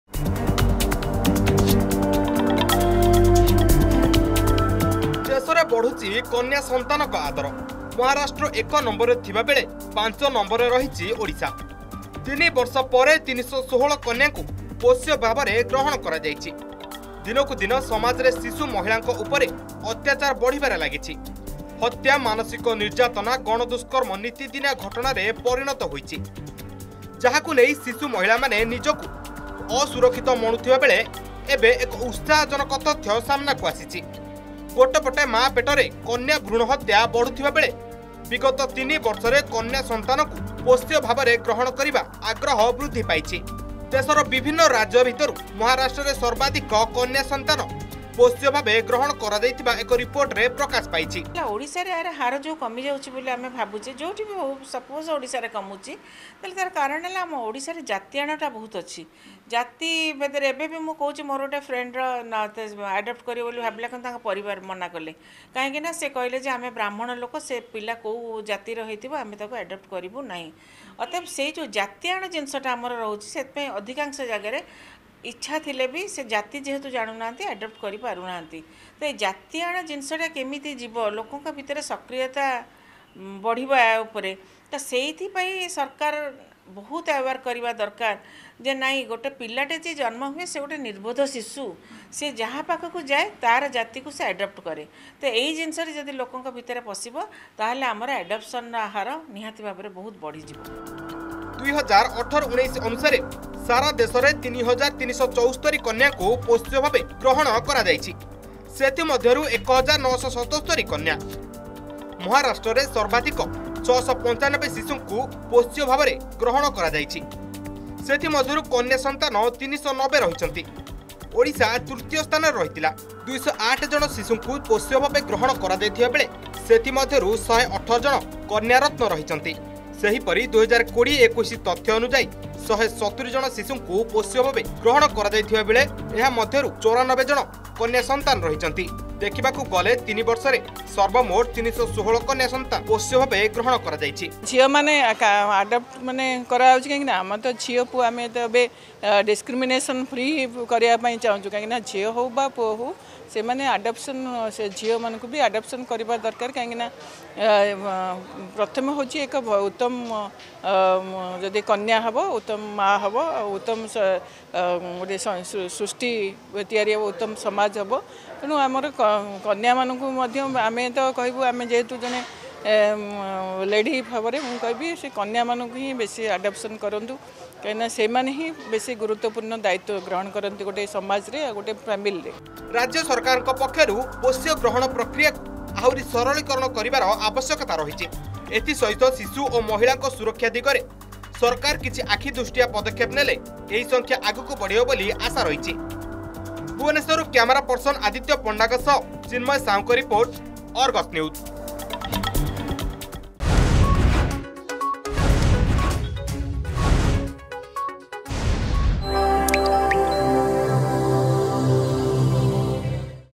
ଦେଶରେ ବଢୁଛି କନ୍ୟା ସନ୍ତାନଙ୍କ ଆଦର
ମହାରାଷ୍ଟ୍ର ୧ ନମ୍ବର ଥିବା ବେଳେ ୫ ନମ୍ବରରେ ରହିଛି ଓଡ଼ିଶା
ଦିନକୁ ଦିନ ସମାଜରେ ଶିଶୁ ମହିଳାଙ୍କ ଉପରେ ଅତ୍ୟାଚାର ବଢ଼ିବାରେ ଲାଗିଛି । ହତ୍ୟା, ମାନସିକ ନିର୍ଯାତନା, ଗଣଧର୍ଷଣ ନିତିଦିନିଆ ଘଟଣାରେ ପରିଣତ ହୋଇଛି । ଯାହାକୁ ନେଇ ଶିଶୁ ମହିଳା ମାନେ ନିଜକୁ ଅସୁରକ୍ଷିତ ମଣୁ ଥିବା ବେଳେ, ଏବେ ଏକ ଉତ୍ସାହ ଜନକ ତଥ୍ୟ ସାମନାକୁ ଆସିଛି ।